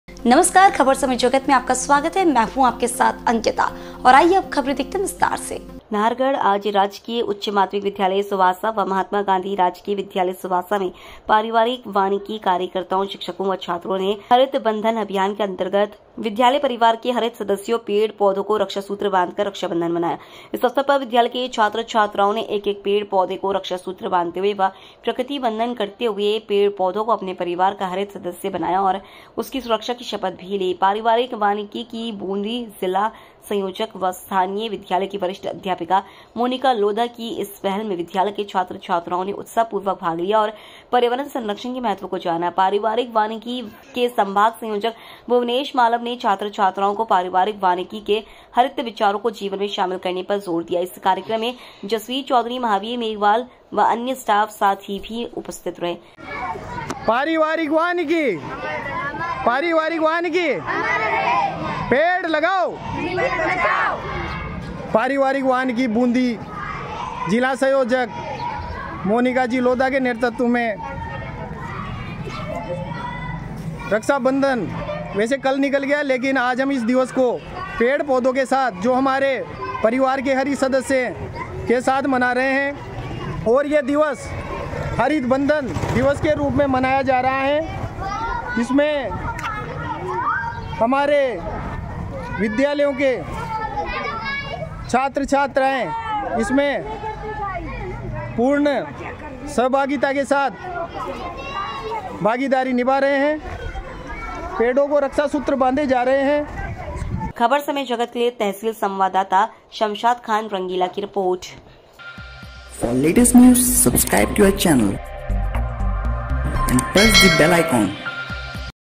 नाहरगढ:- पारिवारिक वानिकी कार्यकर्ताओं ने पेड़ पौधों को रक्षा सूत्र बांधकर मनाया रक्षाबंधन !
संवाददाता शमशाद खान रंगीला